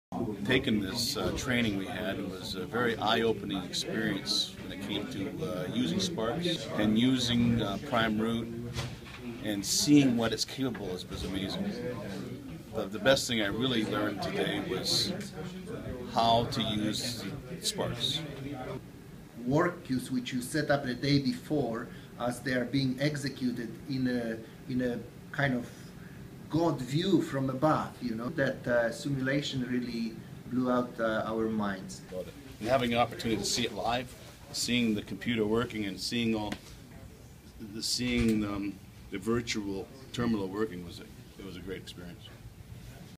Lance & Martin about Virtual Reality Training
Virtual Reality TOS (Terminal Operating System) Training with CONTROLS by TBA
Training was performed at DPWorld Vancouver
Footage agreed by DPWorld Vancouver, Lance and Martin